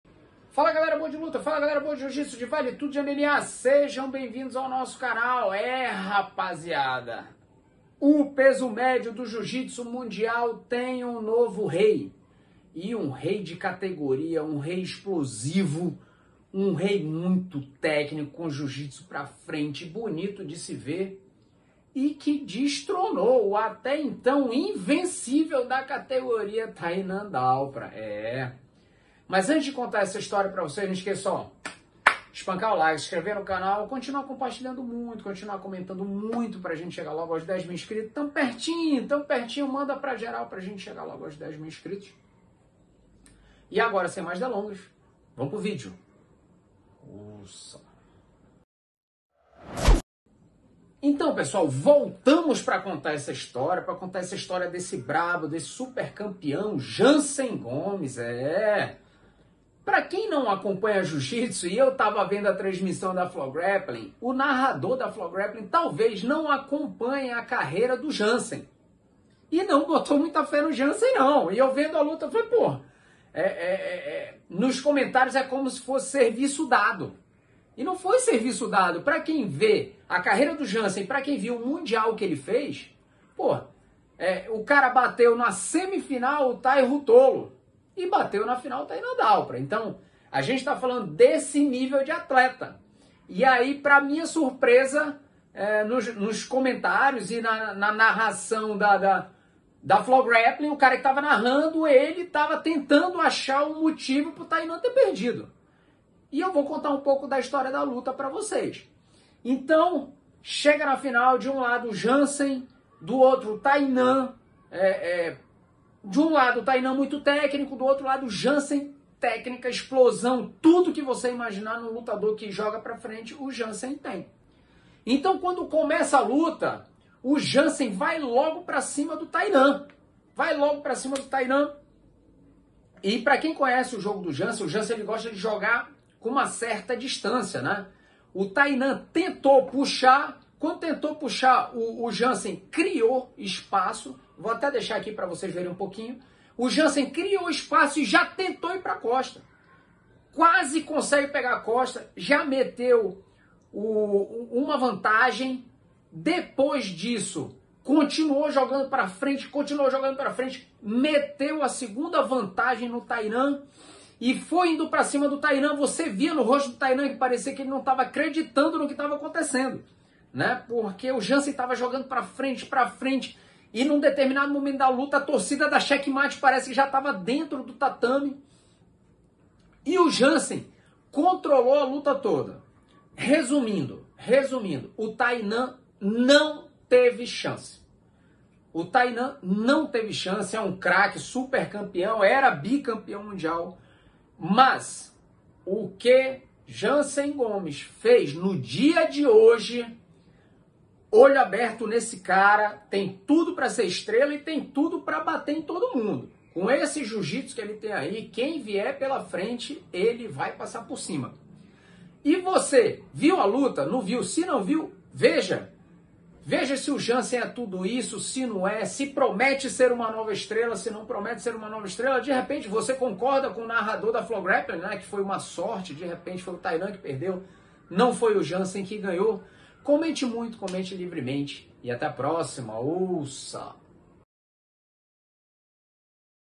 CAMPEÃO MUNDIAL COM UMA DOMINÂNCIA ABSURDA
JANSEN GOMES TRATOROU TODAS AS ESTRELAS DA CATEGORIA MÉDIO E SAGROU-SE CAMPEÃO MUNDIAL INCONTESTÁVEL.